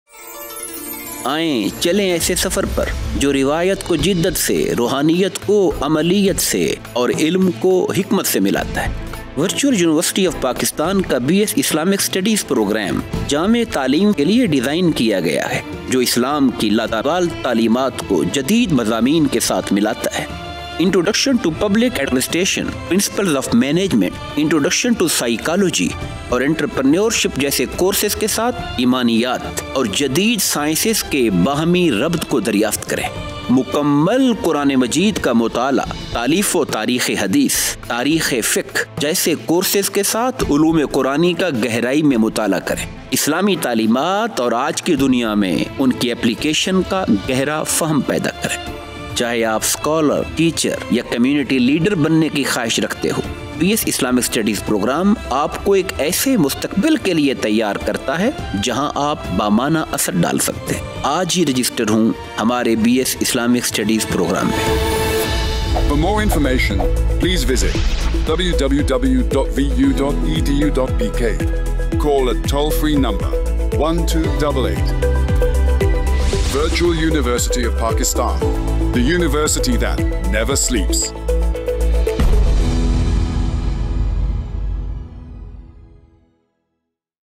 BS Islamic Studies | Admission Open | Virtual University of Pakistan
BS Islamic Studies | Virtual University of Pakistan | Admission Open
Unlock a deeper understanding of Islam with the BS Islamic Studies program at Virtual University of Pakistan. This comprehensive program is designed to offer students a profound knowledge of Islamic teachings, history, and culture through a modern and flexible learning environment.
Whether you're aiming to pursue a career in education, research, or simply wish to enrich your understanding of Islam, this program is tailored to meet your academic and spiritual needs. With admissions now open, it's the perfect time to start your journey towards a rewarding and meaningful education.

Key Highlights:
Flexible online classes
Expert faculty with extensive knowledge in Islamic Studies
Access to a wide range of digital resources and libraries
A curriculum that blends traditional Islamic knowledge with modern perspectives
Join thousands of students who have chosen Virtual University for its quality education and accessible learning. Don't miss this opportunity to enhance your knowledge and skills.

Apply Now!